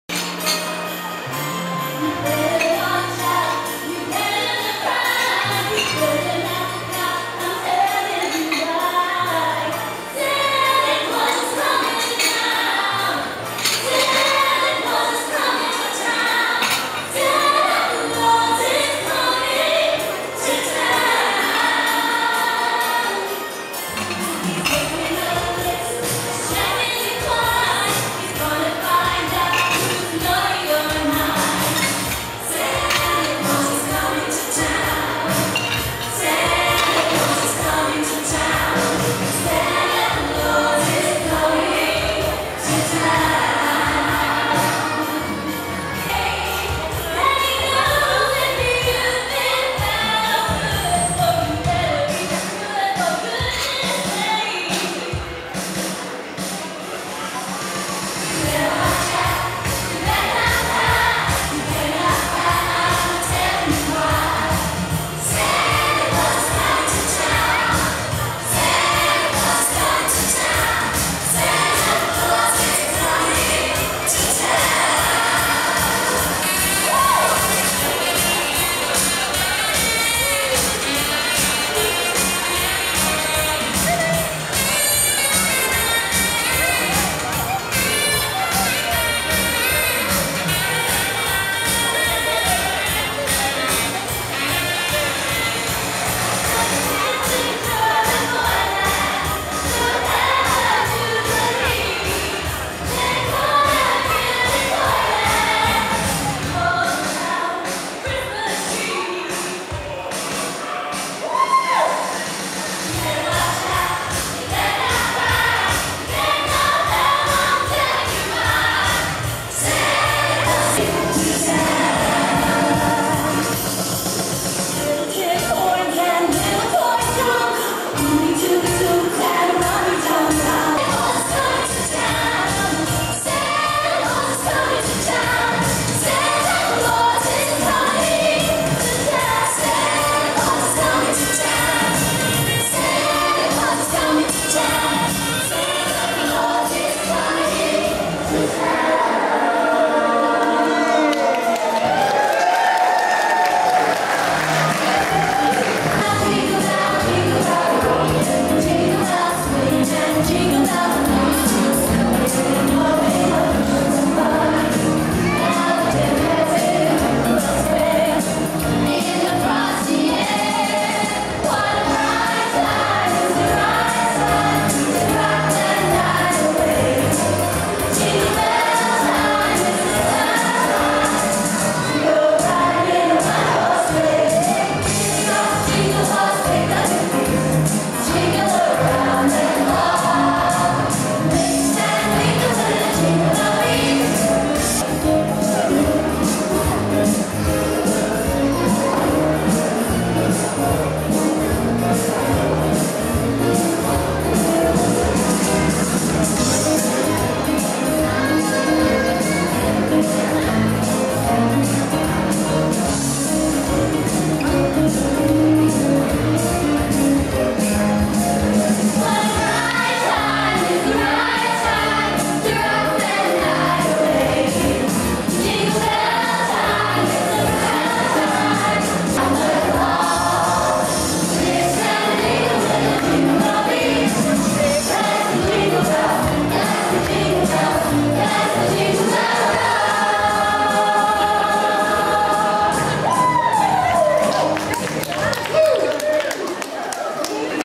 Flash Mob at LYNNMALL
On the 17th of November 2012, a FLASHMOB broke out in Centre!